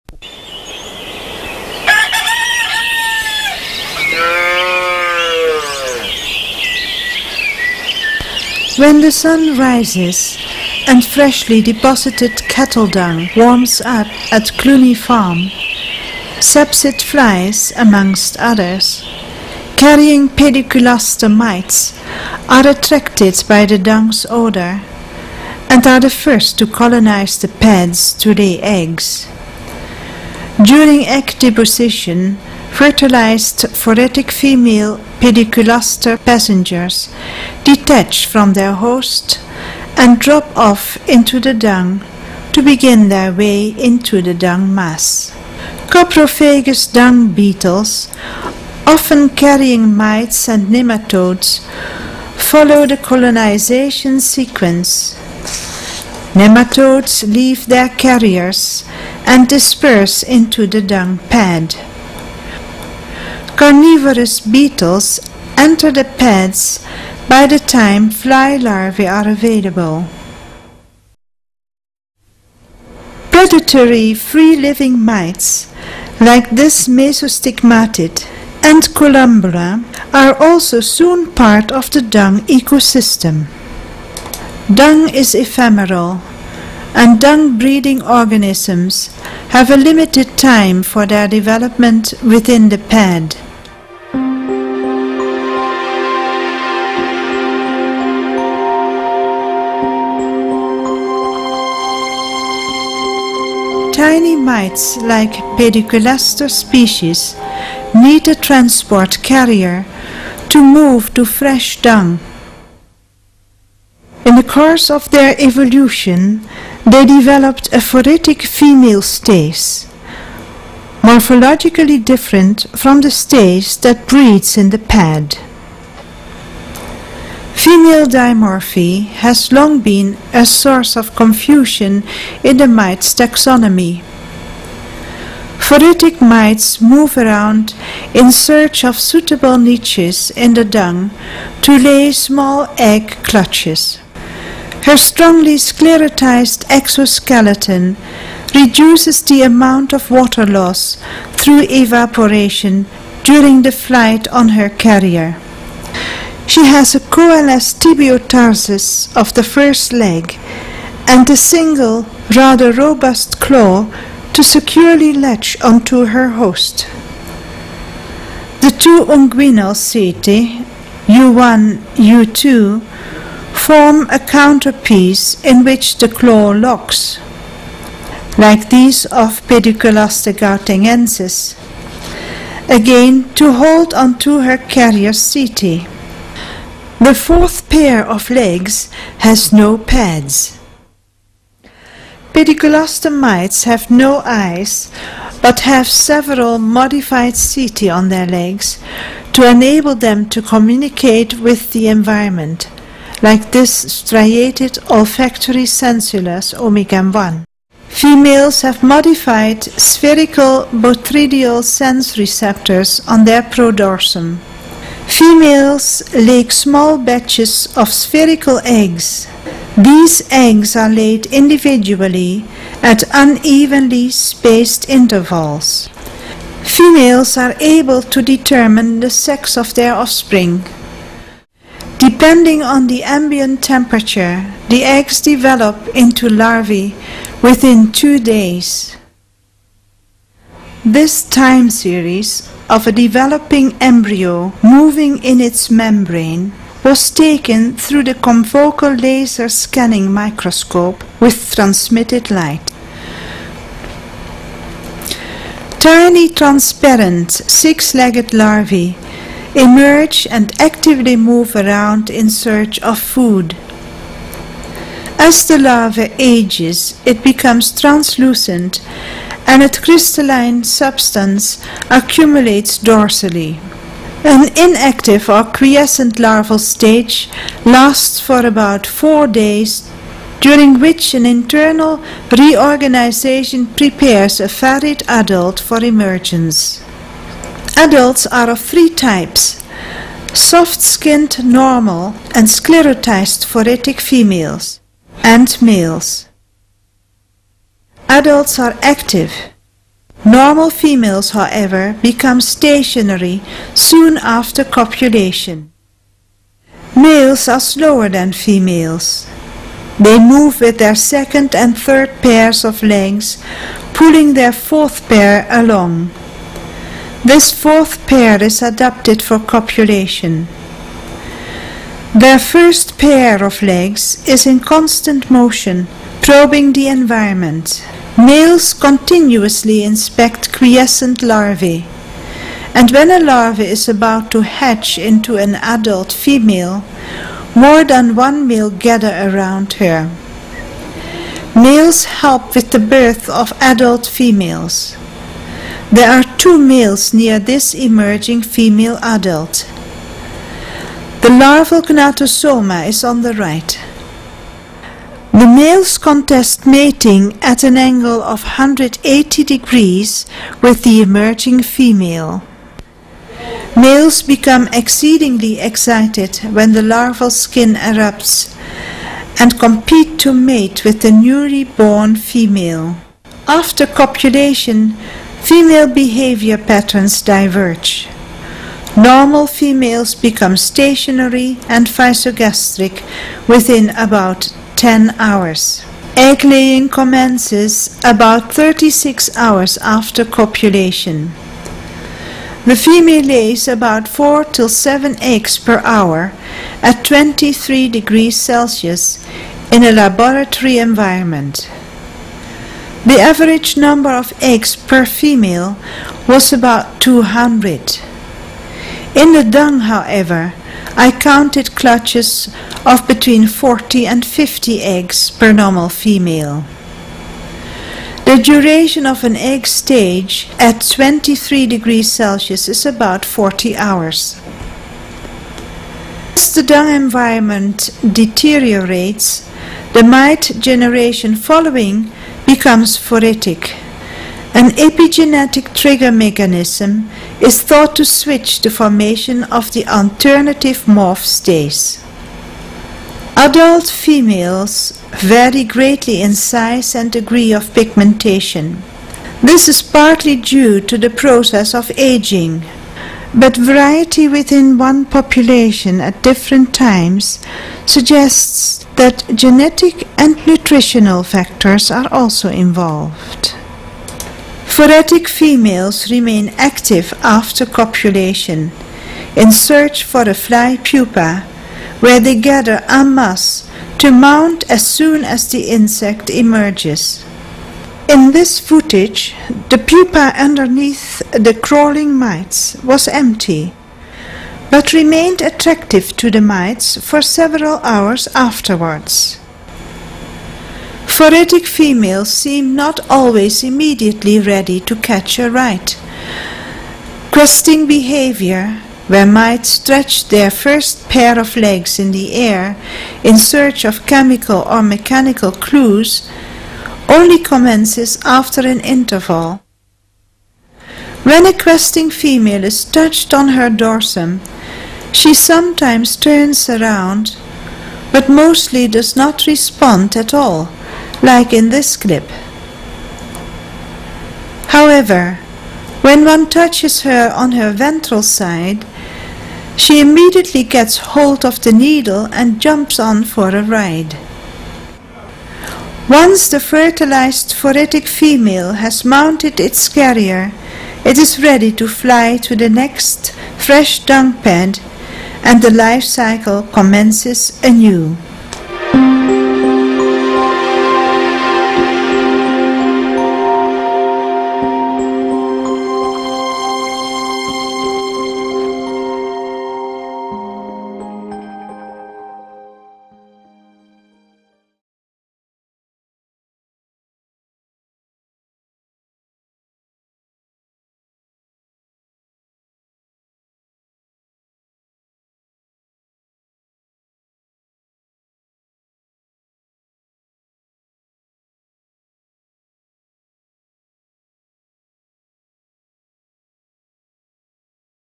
Life cycle of Pediculaster (Acari: Siteroptidae) in cattle dung
The interesting life cycle of Pediculaster sp (Acari: Siteroptidae) in cattle dung in South Africa is presented by Anne M. Camerik. These mites are fungivorous and live in soil, litter, mosses, dung, nest material, and various plants and fruits. Phoretic stages of these mites may be found on flies and other insects.